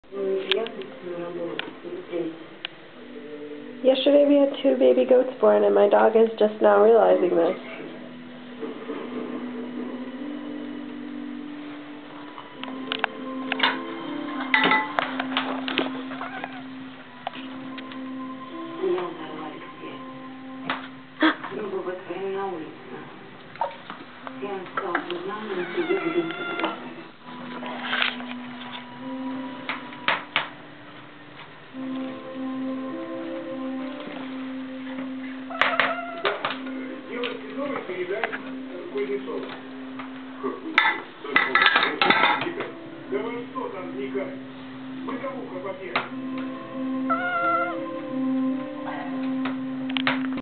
Vanessa meets the baby goats
Cute interaction between my puppy and one-day-old baby goats.  Their names are Sara and Mara.